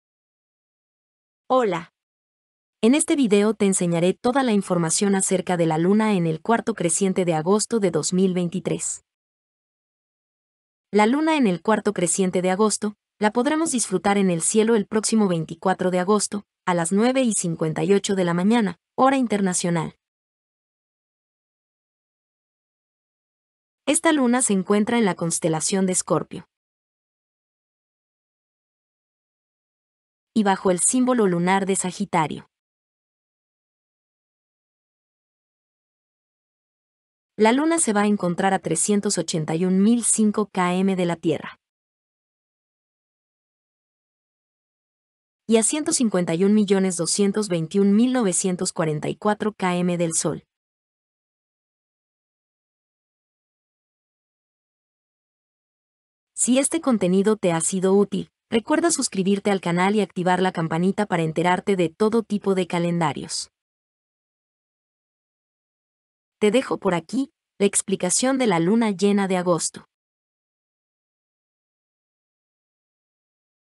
▶ EXPLICACIÓN ASTROLOGÍA LUNA EN EL CUARTO CRECIENTE AGOSTO 2023 ✅ DIA HORA CONSTELACIÓN SIGNO LUNA
Entra y descubre la explicación de la luna en el cuarto creciente de agosto de 2023. Descubre el día, la hora, la constelación, el signo, planetas y distancias de la luna a la tierra y del sol a la luna.

LLAVERO:
Llavero luna

LIBRETAS:
Libreta luna llena
Libreta luna llena con frase